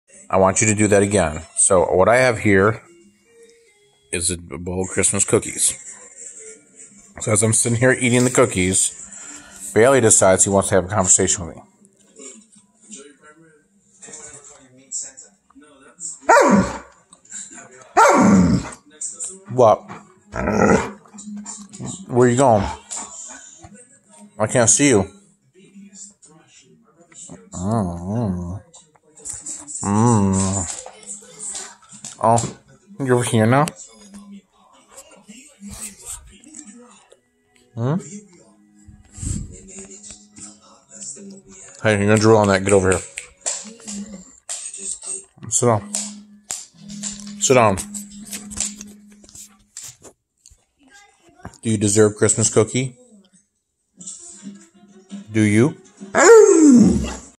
Alaskan Malamute Life: Bailey asks for Xmas Cookies
Bailey asks unprovoked for Christmas cookies